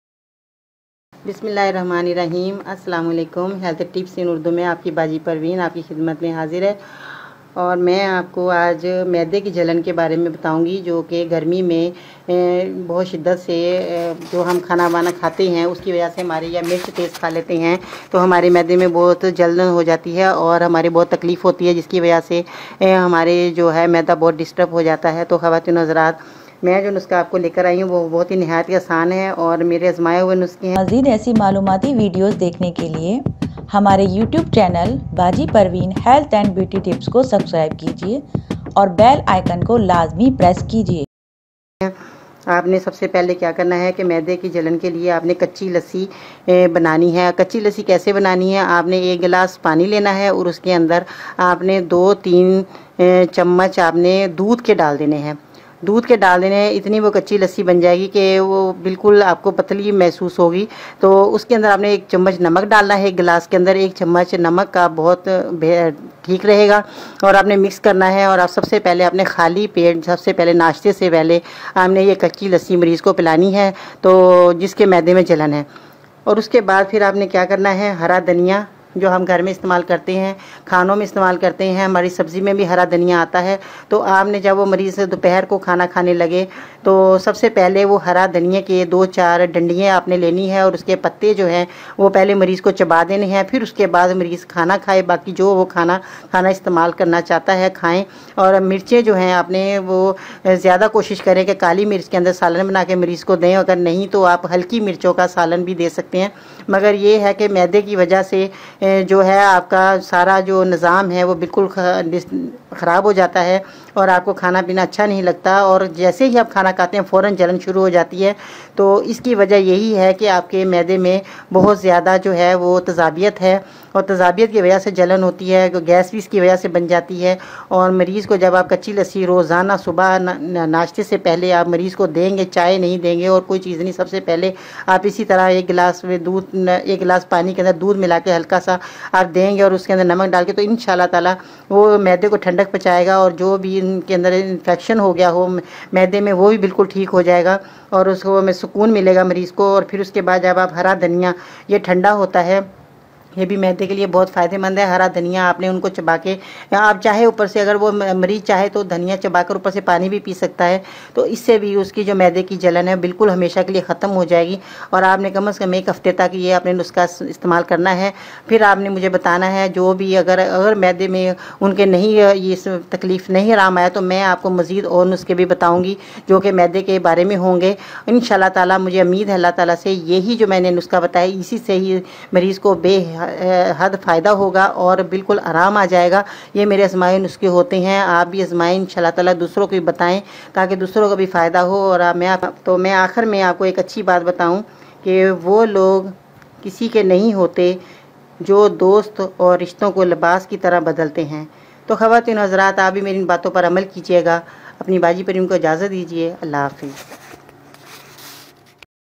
Stomach Burning Treatment - Maidy Ki Jalan Ka Ilaj - Mady Ki Garmi Door Karen
You might have burning or pain in the upper part of your stomach, too. It's indigestion, also called dyspepsia. Indigestion is often a sign of an underlying problem, such as gastroesophageal reflux disease ulcers, or gallbladder disease, rather than a condition of its own.

Ingredients
Water
Salt
Milk

Take 1 Glass Water Add 2 Spoon Of Milk In this Water Also Add Some Salt Mix It Well And Drink It Before Breakfast.